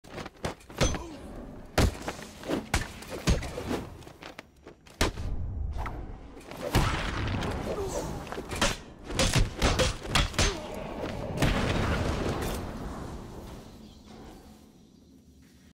Robin causes severe damage to thug, then has a stroke
That looked like it hurt.